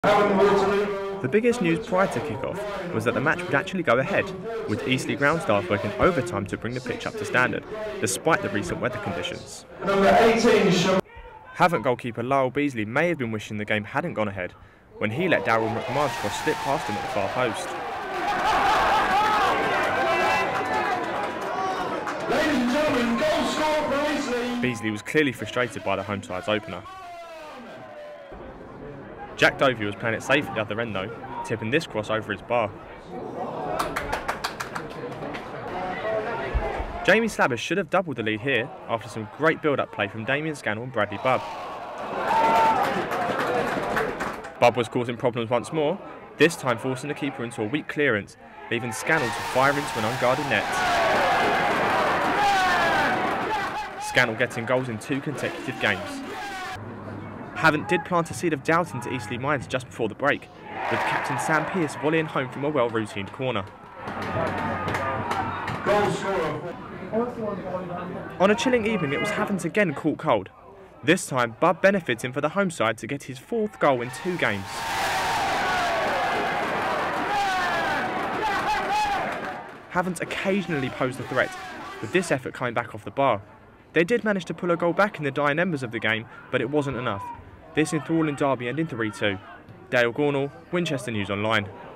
Eastleigh Vs Havant and Waterlooville
Eastleigh take on local rivals Havant and Waterlooville in the Blue Square South. Dael Gornall reports.